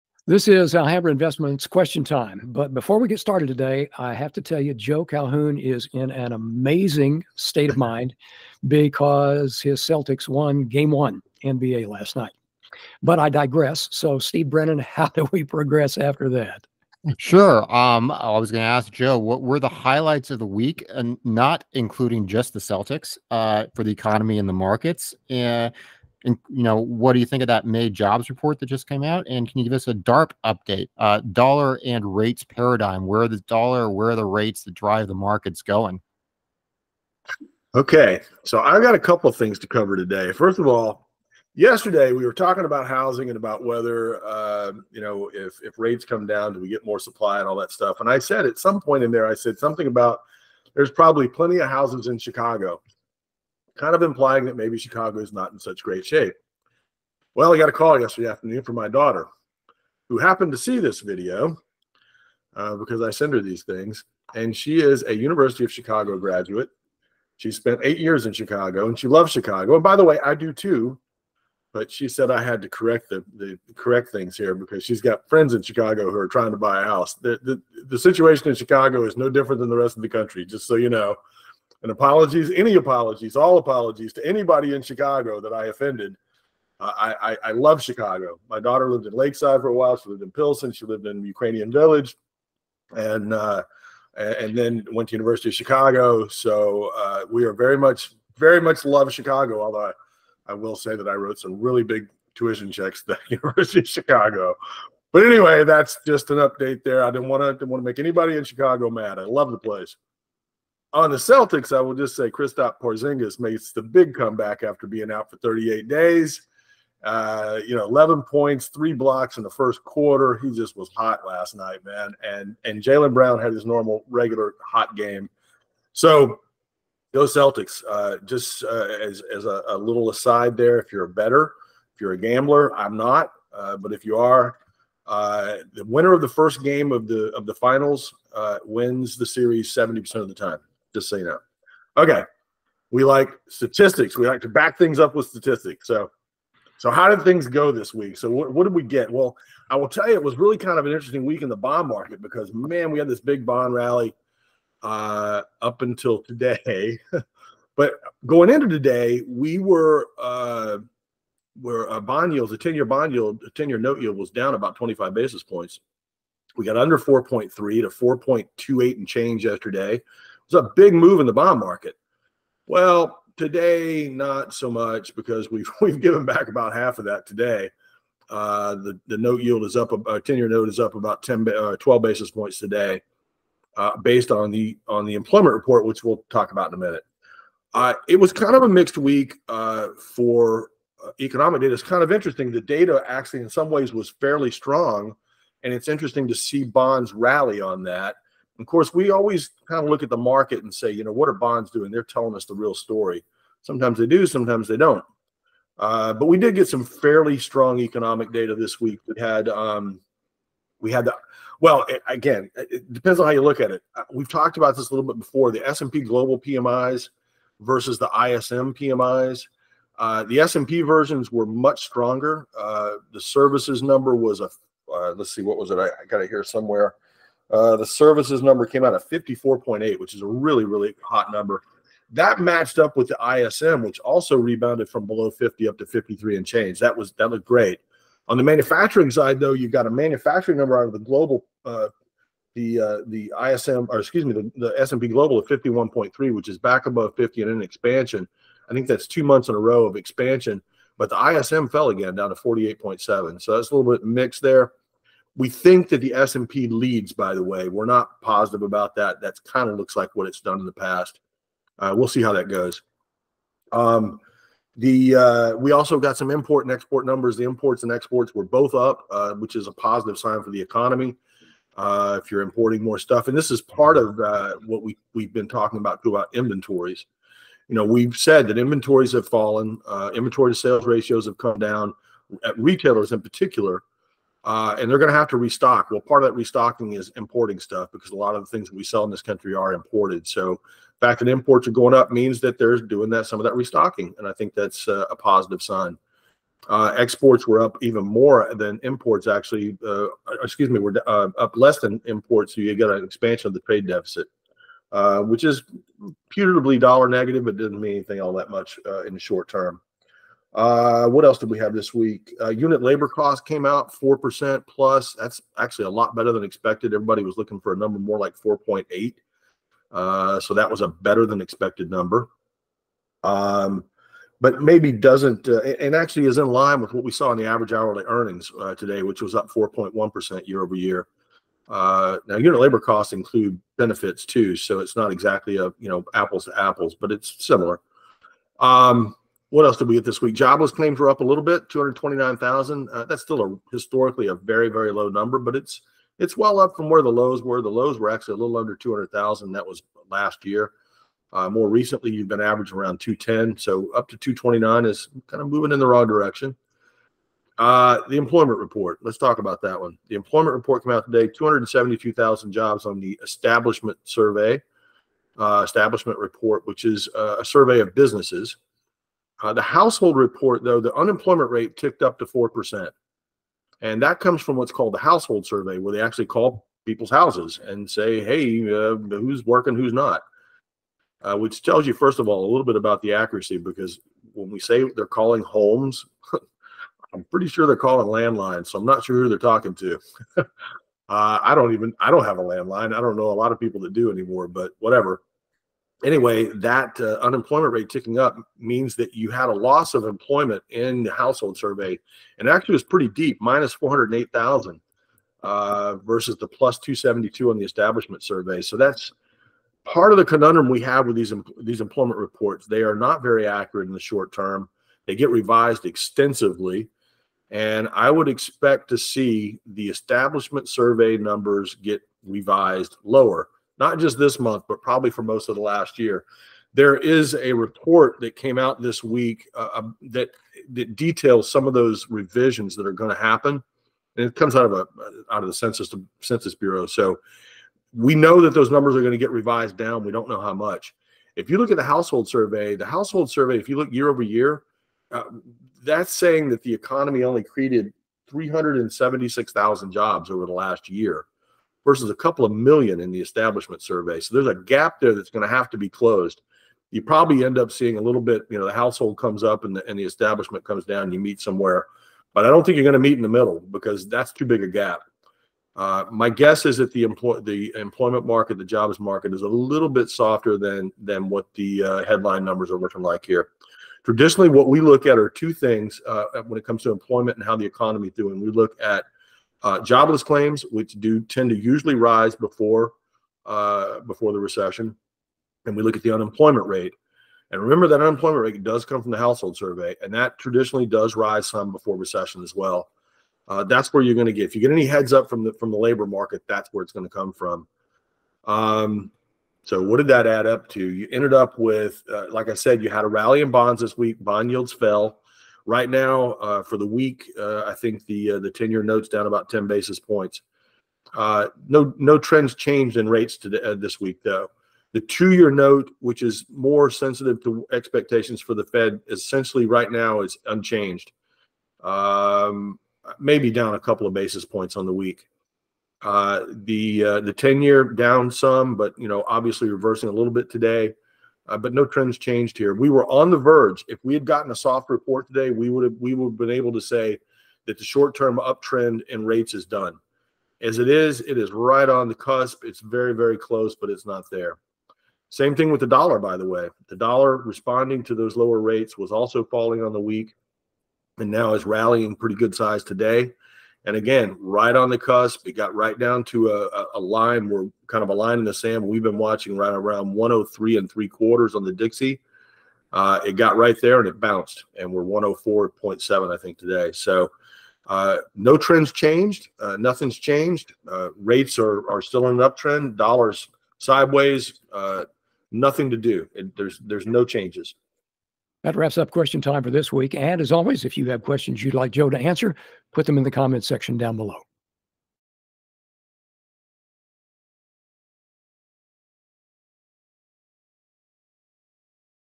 Stock Market and DARP Update
Weekly update on financial markets, May jobs report and other economic data with Alhambra CEO Joe Calhoun.